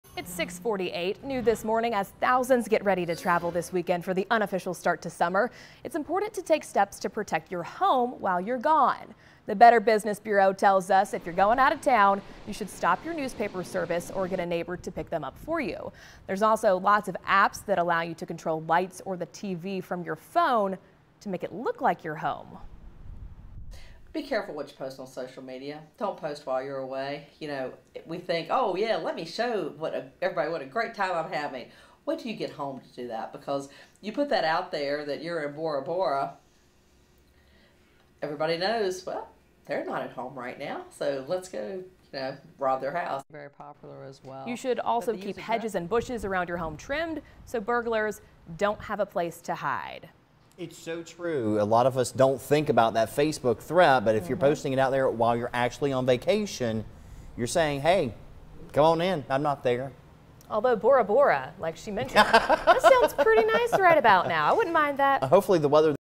Summer vacation scams: Be careful what you post on social media
Posting vacation photos while you're still on vacation lets people know you are not home.